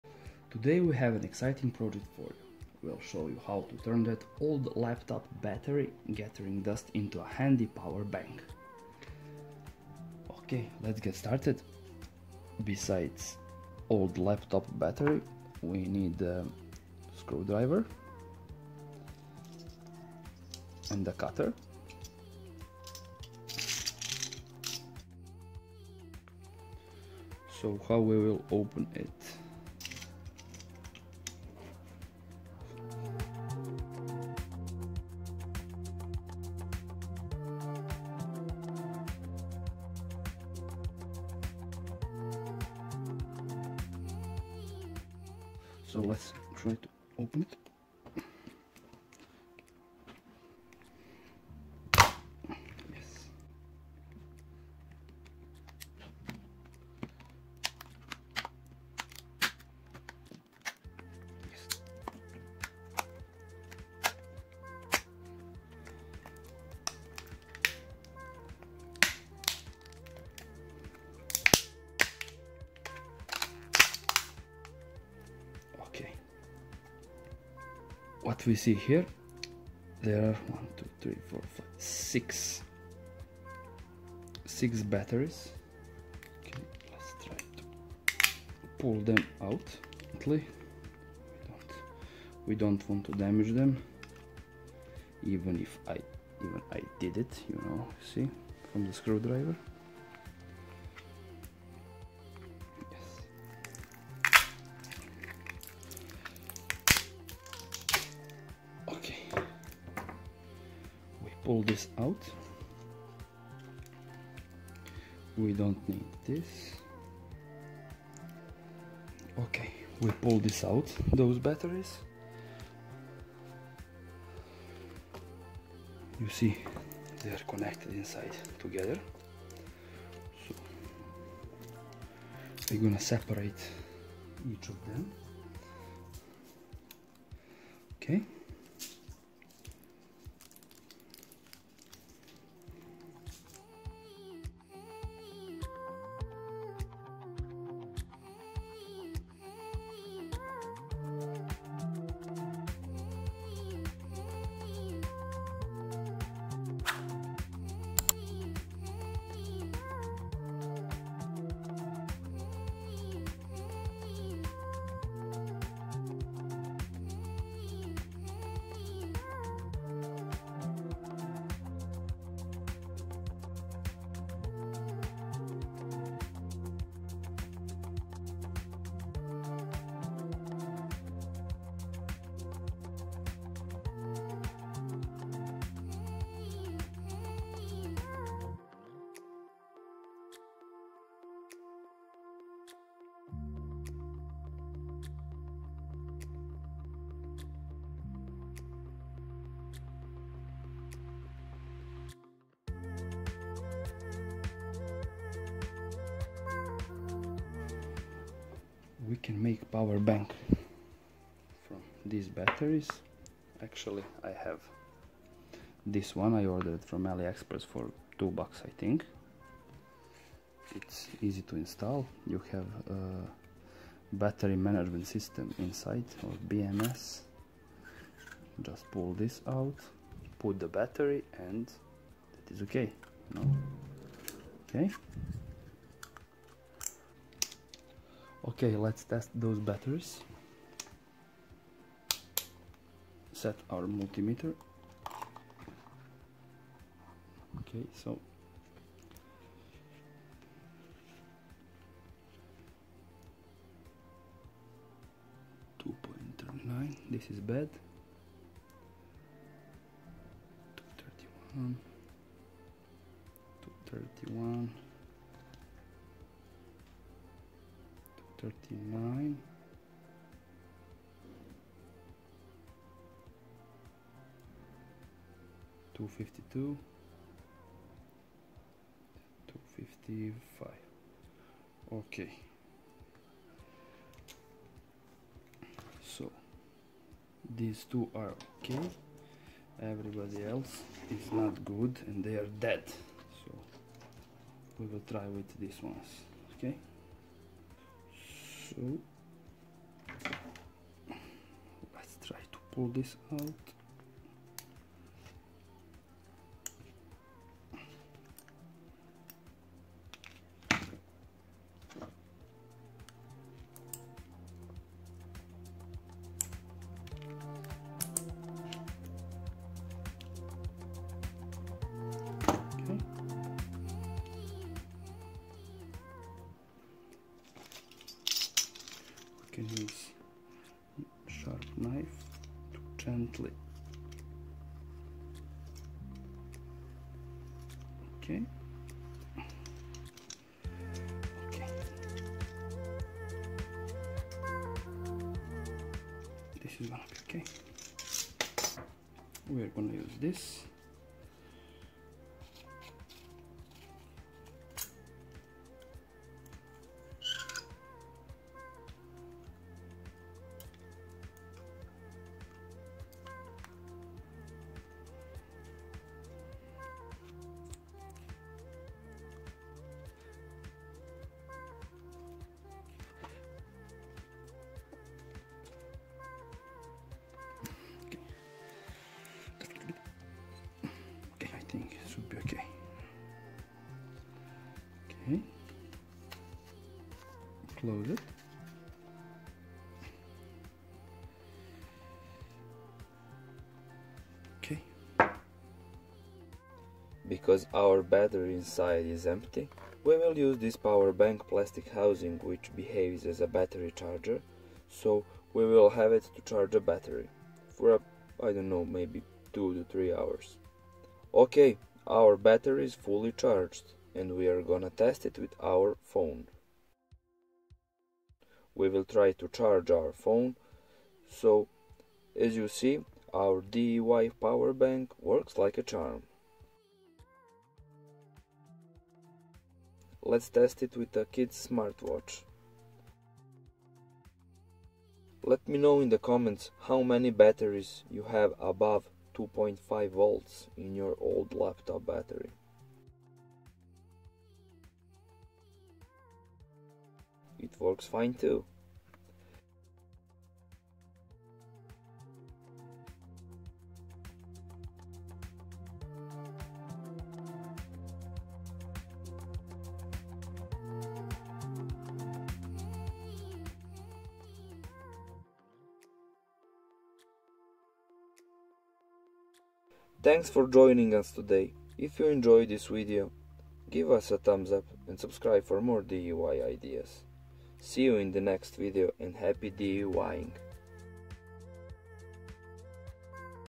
Turn Your Old Laptop Battery into a Portable Power Bank!
Today you will learn how to turn your old laptop battery into a portable power bank! Instead of throwing away your old laptop battery, you can repurpose it and create a portable charger that you can take with you anywhere. Not only is this a great way to save money, but it's also an eco-friendly solution that reduces waste.

Our step-by-step guide will show you how to safely extract the battery from your laptop and turn your old laptop battery as a power bank. With this DIY power bank used from old laptop battery, you'll never have to worry about running out of battery on the go again. Plus, you'll have the satisfaction of knowing that you've repurposed an old item and given it new life.

So, grab your old laptop battery and let's get started on this fun and practical project!